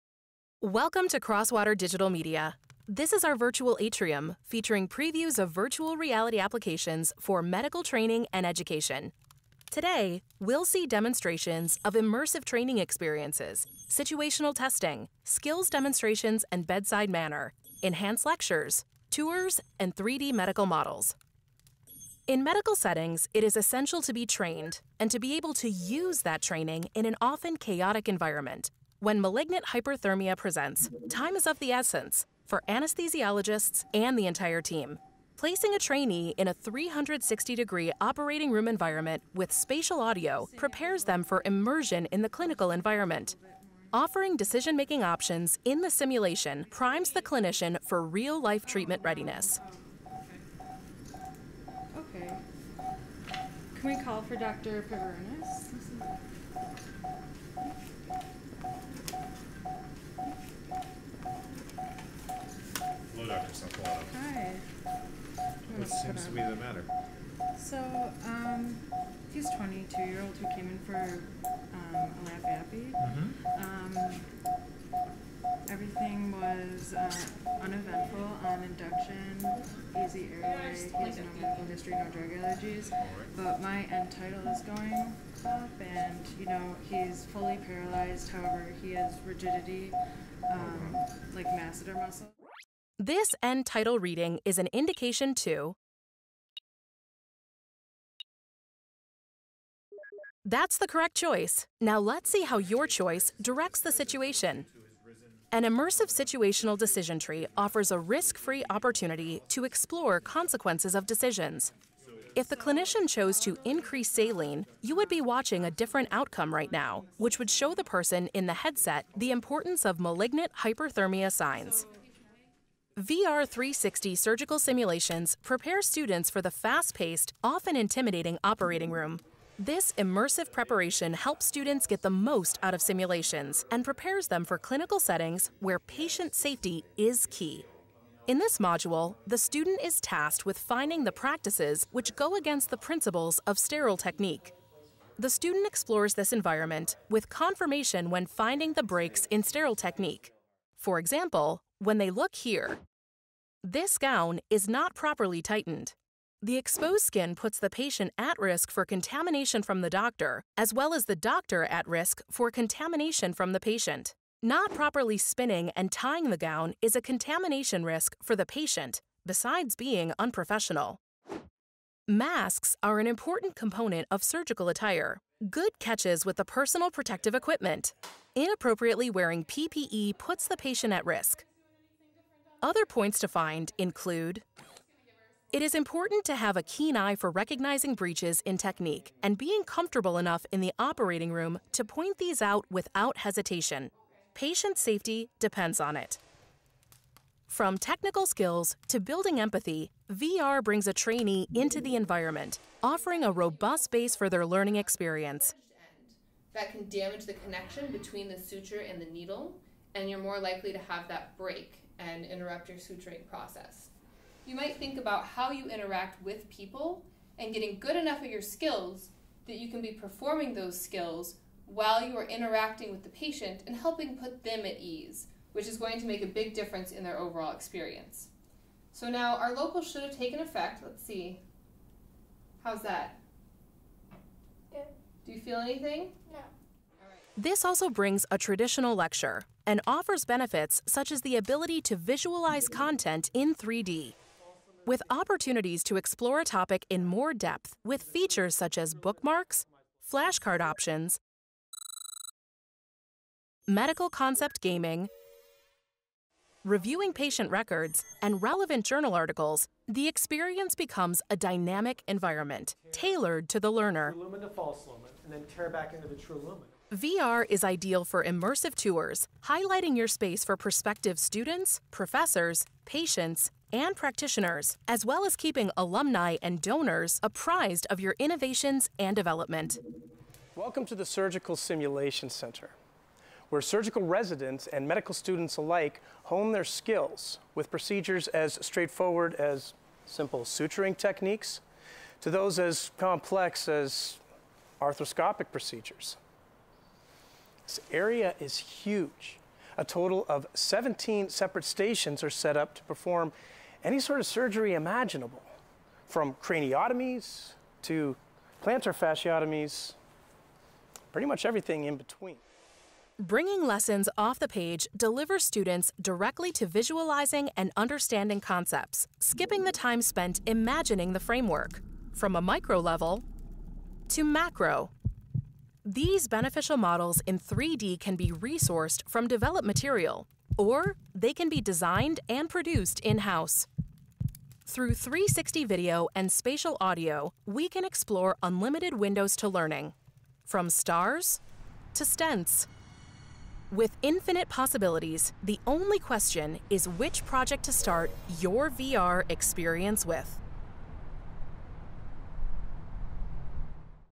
Crosswater Medical VR/360 for Training and Education Simulation
Spark your imagination and explore the possibilities with Crosswater's VR/360 for Medical education and training applications.